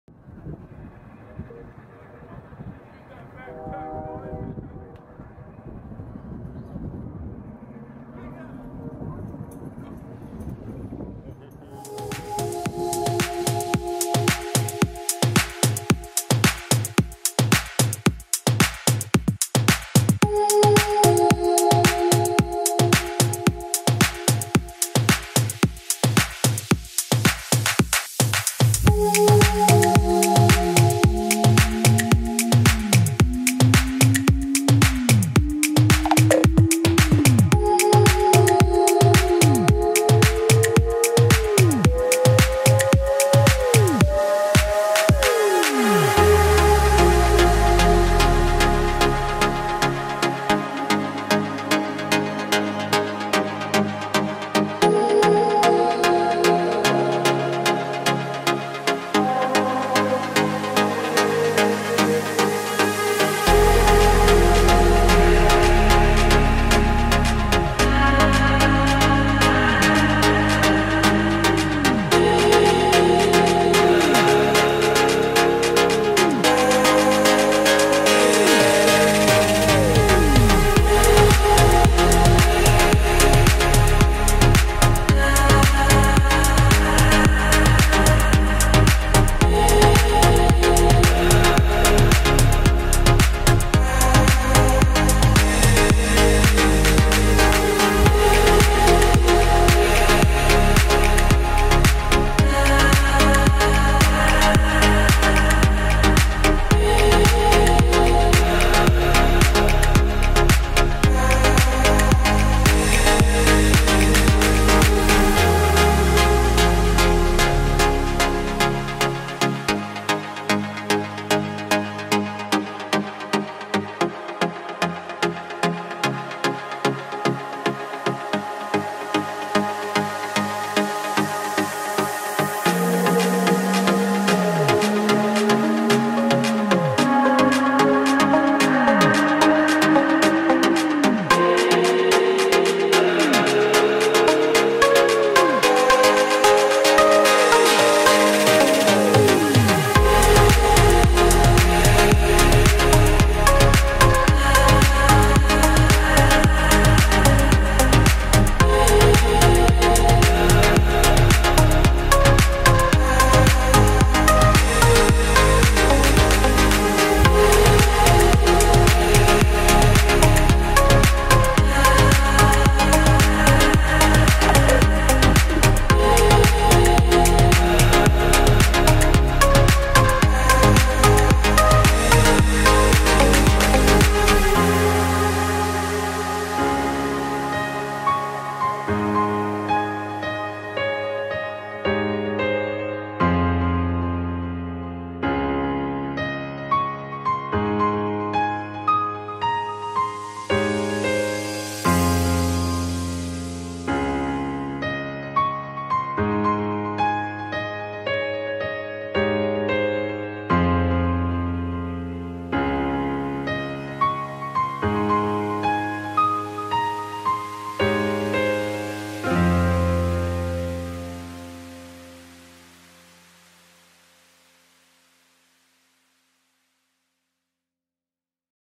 River Fire
Poetic movements with fire, sunset scenery, and upbeat music from the other side of the Atlantic Ocean.

Fire-Poi artists: Cody Cochran Jake Dolan Garrett Hayes Alexis Wright Corey Cochran Carlos Vallejo Emma Wright
Location: Arkansas River bank in Tulsa, Oklahoma